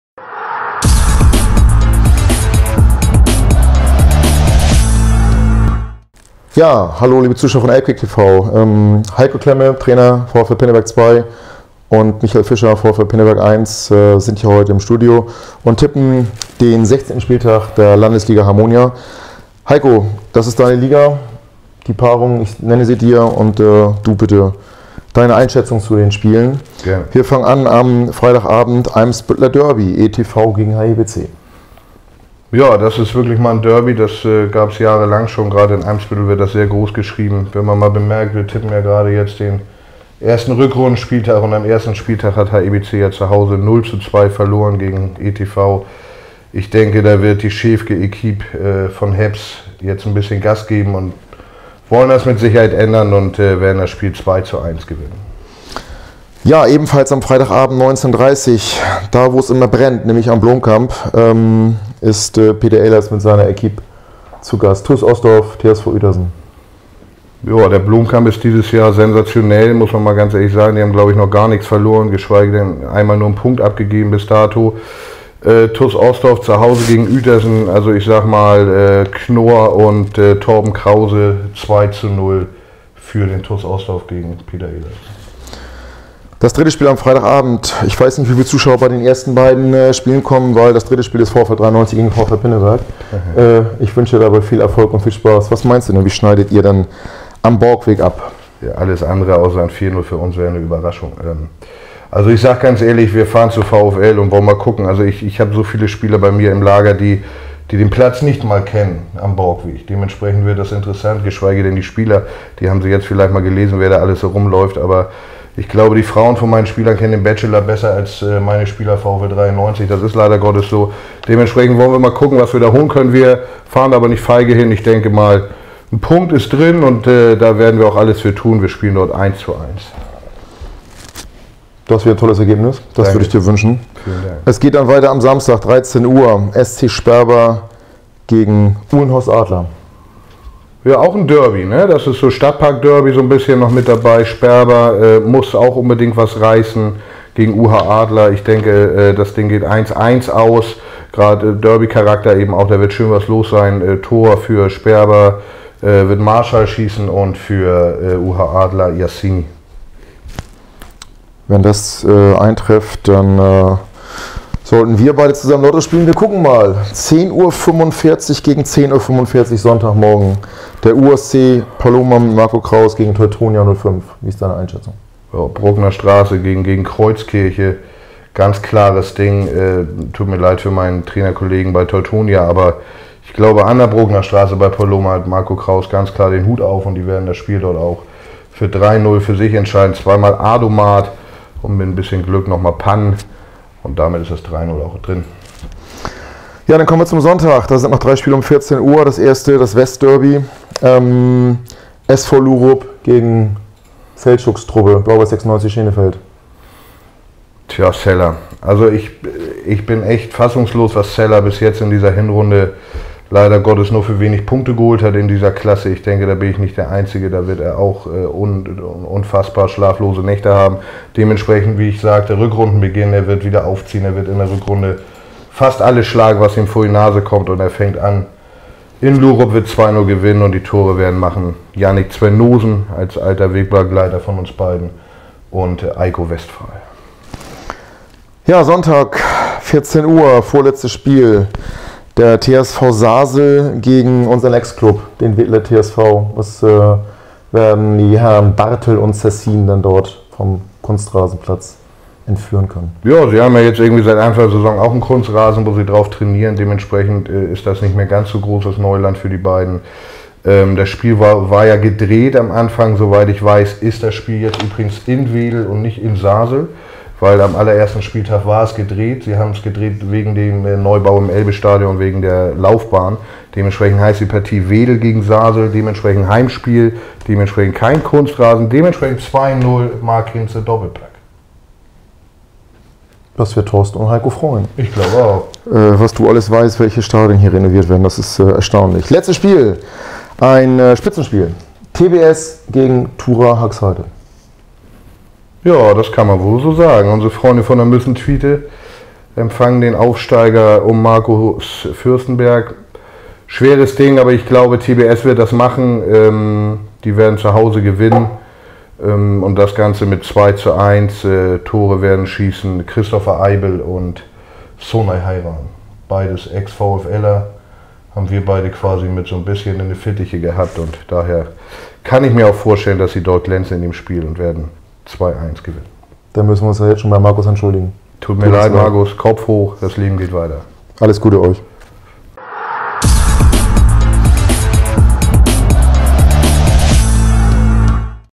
Michael Fischer und Heiko Klemme tippen den 16. Spieltag der Landesliga Hammonia | ELBKICK.TV
Michael Fischer und Heiko Klemme (VfL Pinnrberg) tippen den 16. Spieltag der
Landesliga Hammonia. Stimmt ihr zu oder liegen sie daneben?

Wenn auch Ihr Eure Fachkenntnis zeigen und einen Spieltag
oder schreibt uns bei Facebook.